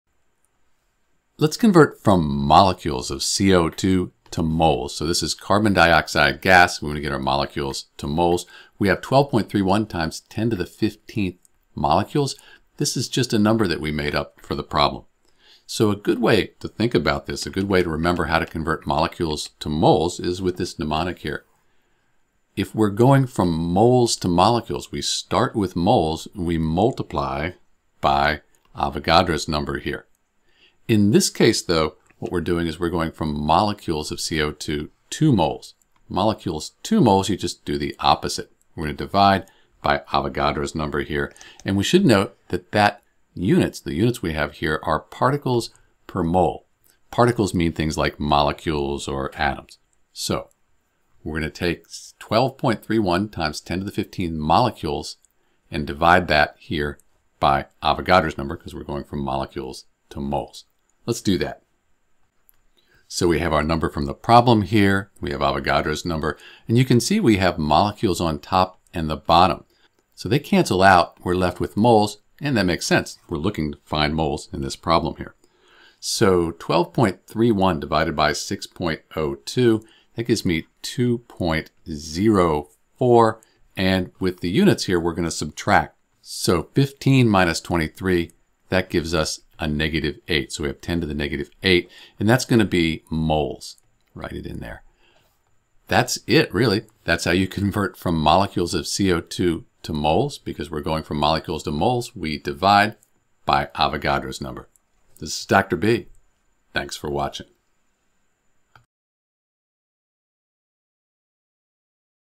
Convert Molecules CO2 to Moles
To convert 12.31 x 10^15 molecules of CO2 to moles, we use Avogadro's number, which is approximately 6.022 x 10^23 molecules per mole.

Step 1: Determine the conversion factor
1 mole CO2 = 6.022 x 10^23 molecules CO2

Step 2: Set up the conversion
12.31 x 10^15 molecules CO2 * (1 mole CO2 / 6.022 x 10^23 molecules CO2)

Step 3: Calculate the result
12.31 x 10^15 / 6.022 x 10^23 ≈ 2.04 x 10^-8 moles

Step 4: Determine the correct significant figures
The original number, 12.31 x 10^15, has four significant figures, so the final answer should also have four significant figures. Therefore, the result is approximately 2.04 x 10^-8 moles of CO2.

For more practice and a better understanding of moles, grams, and other related concepts, you can check out the following resources:

For further resources and information related to chemistry, visit my chemistry